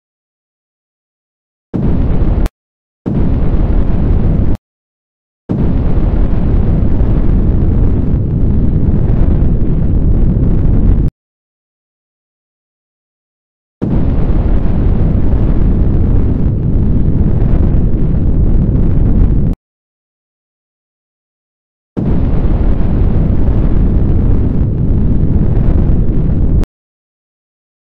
Flamethrower Unity3D
Using particle collisions to send damage message to a health carrier.